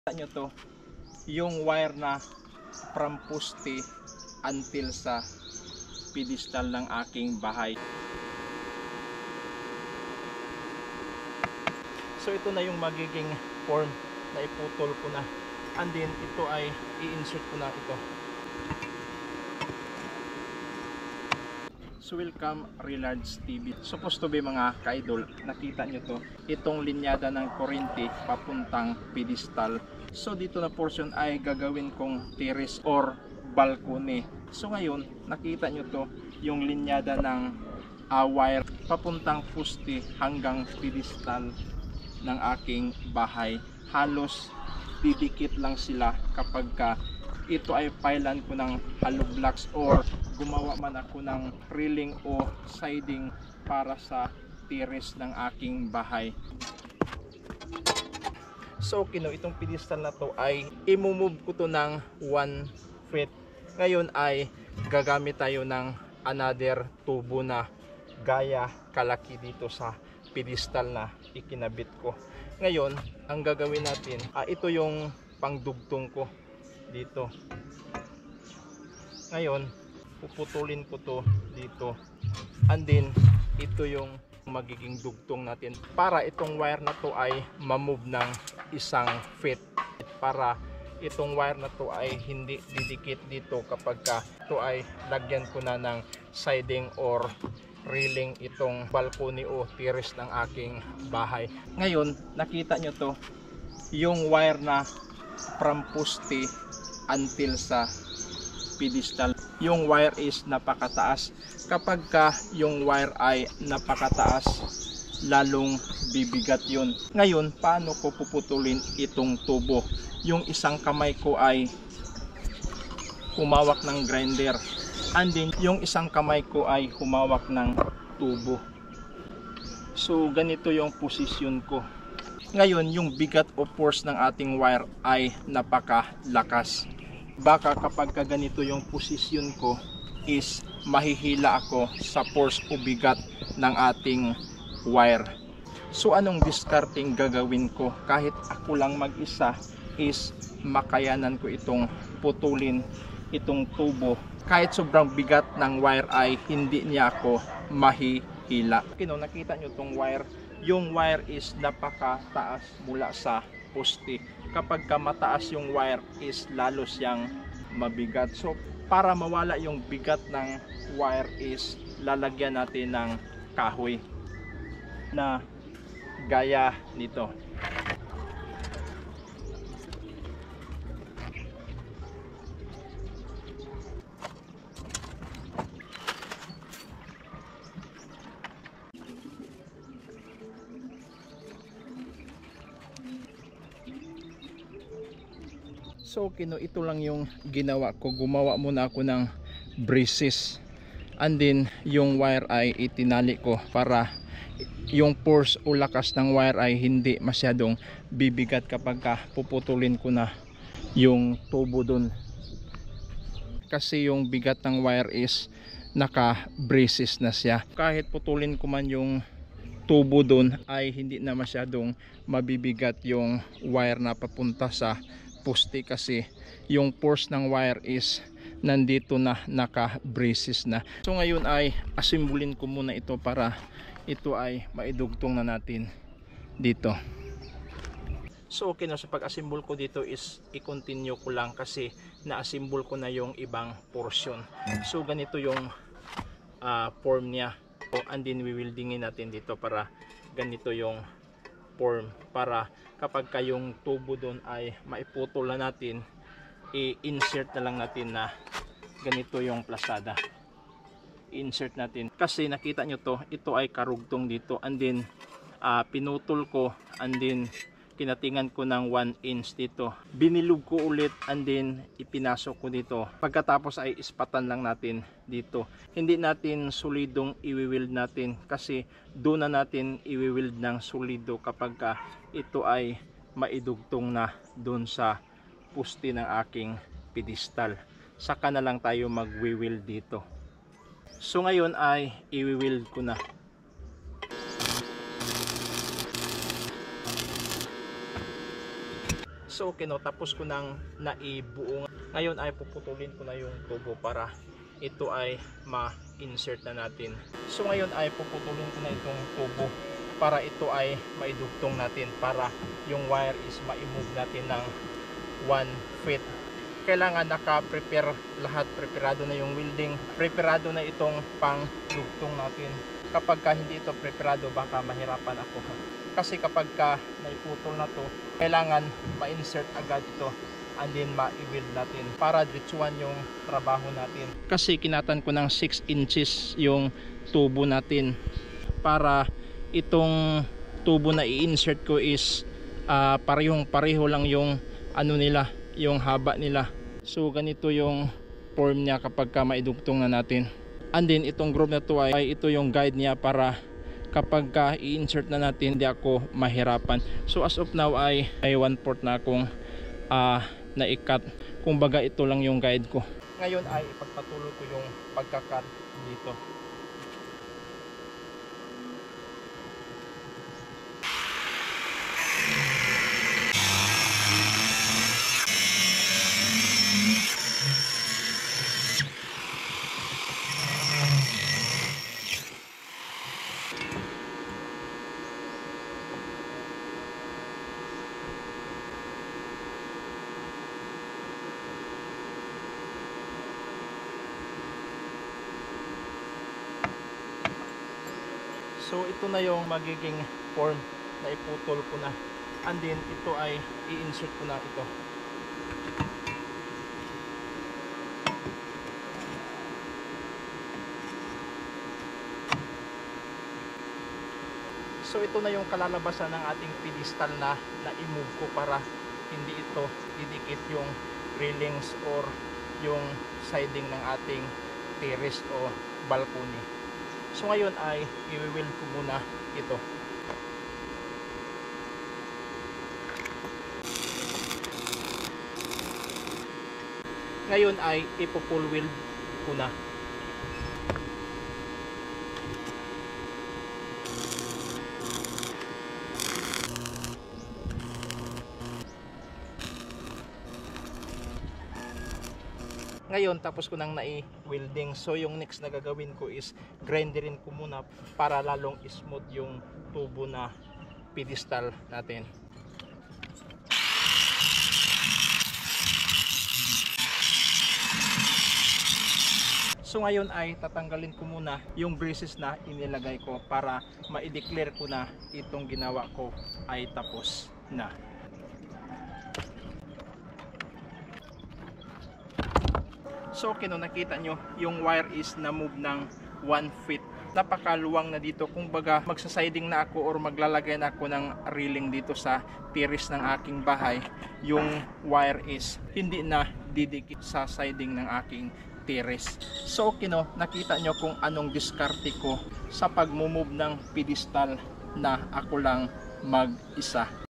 paano putulin at dugtungan pedestal poste na mag isa para hindi sasayad ang wire
paano gumawa ng bahay na walang gasto sa labor kahit peso